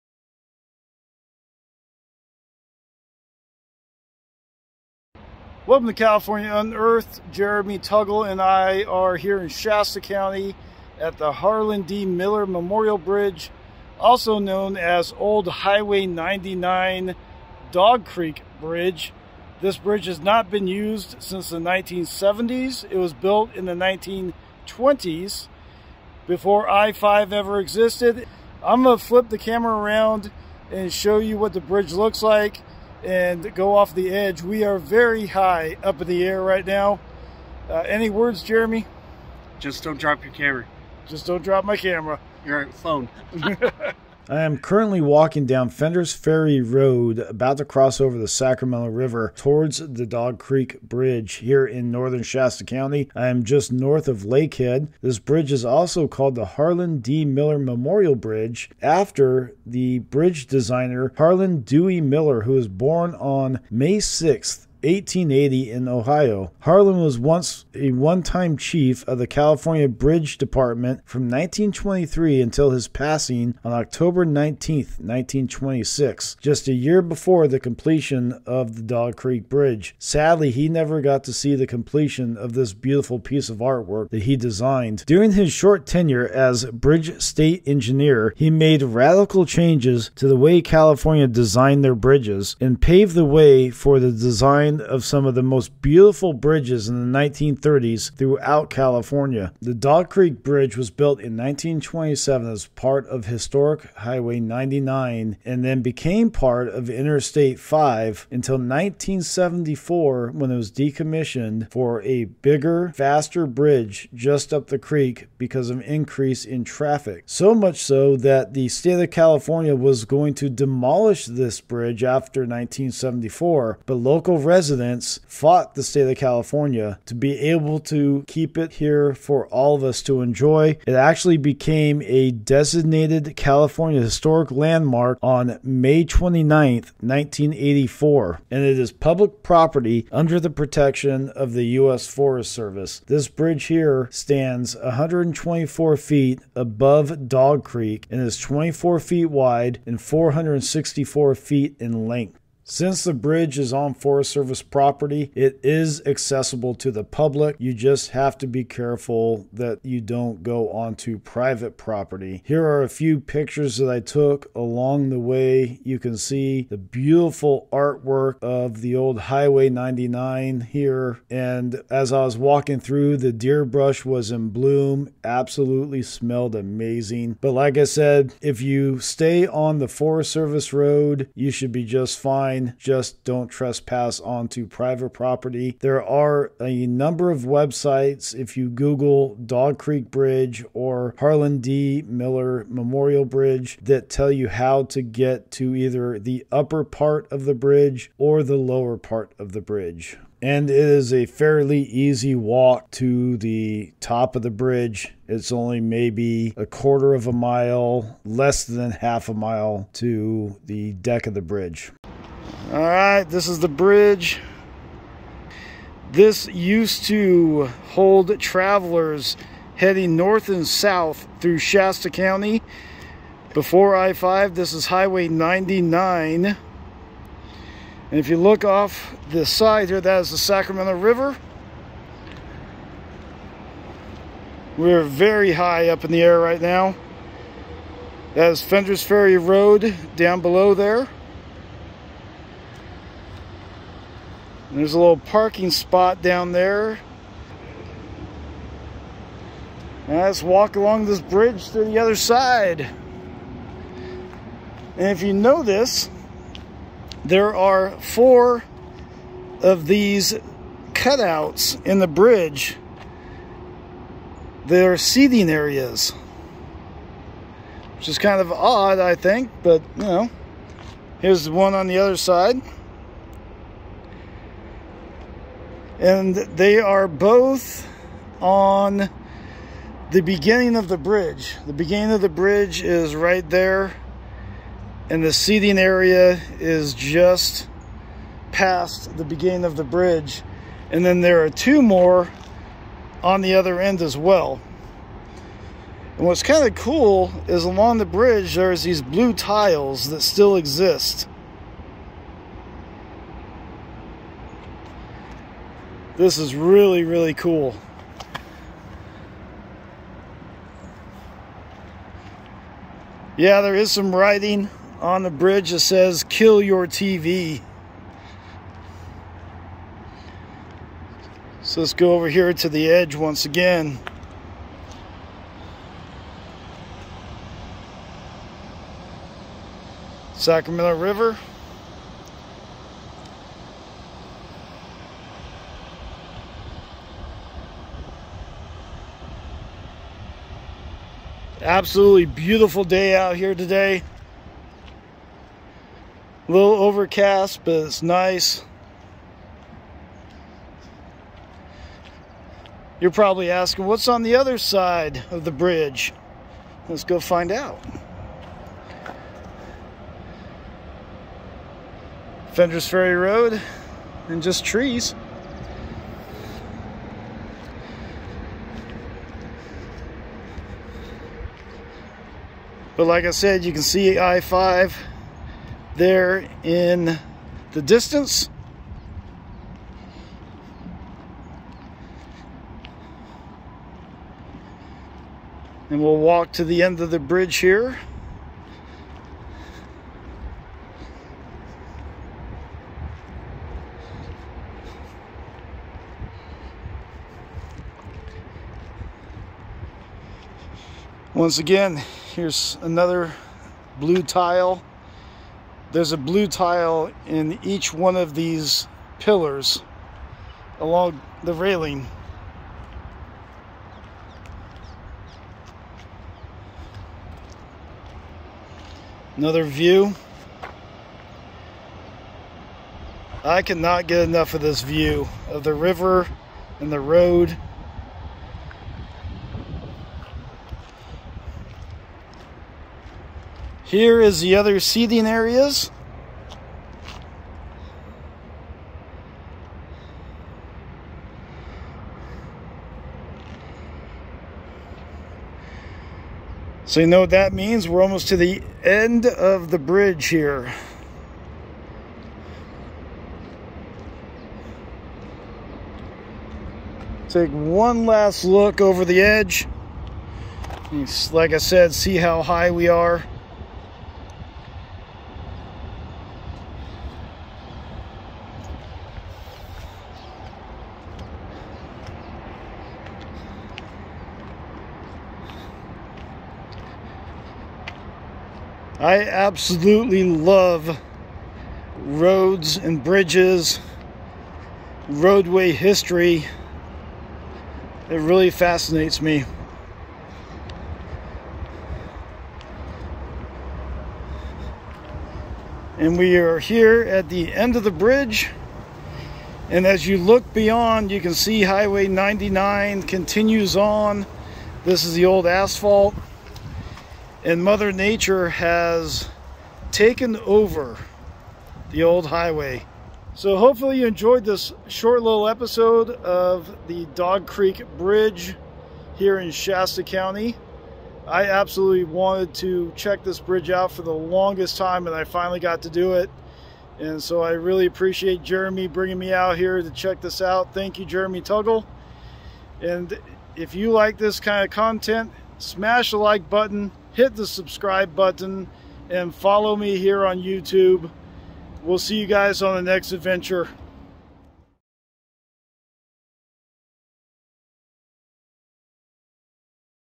Unearthing Old Historic Hwy 99 Dog Creek Bridge
Exploring old Historic Hwy 99 Dog Creek bridge also known as the Harlan D. Miller Memorial Bridge In the Northern part of Shasta County. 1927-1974